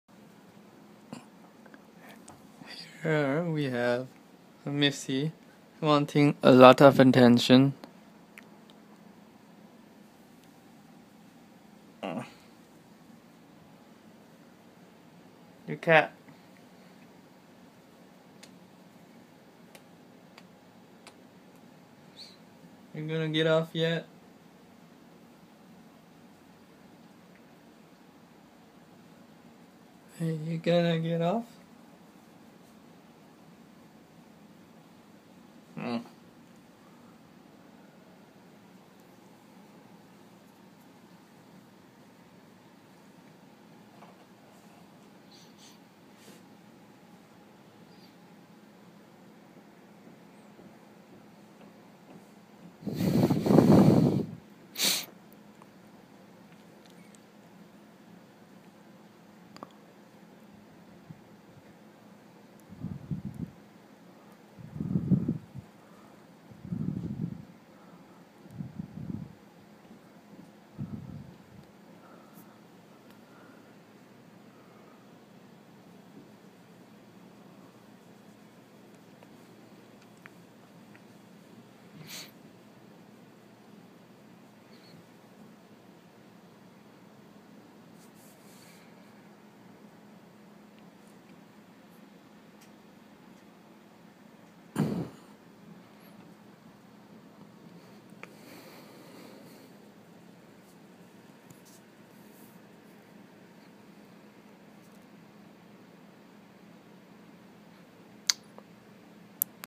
the snuggle baby
missy likes to snuggle like a baby when shes full (food)/sleepy/happy